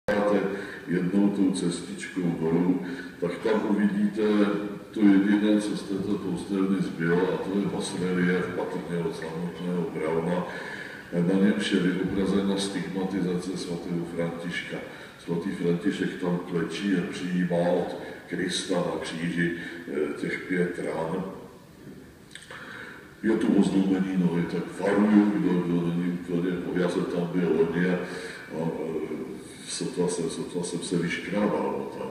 Holographic film CLEAR - new film type - more transparent!
Rear holographic film UGO! CLEAR used for another "ghost" projection within the Night of the Churches 2014 (Kohoutov). We have used a brand new version of the film with higher transparency and greater contrast.